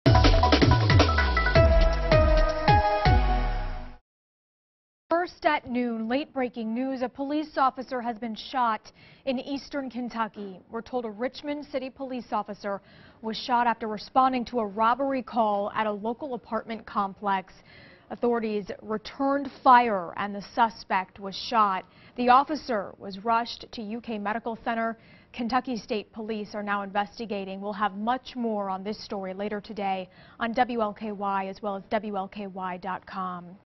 Richmond police officer shot in head
A Richmond police officer was shot in the head Wednesday during a robbery call.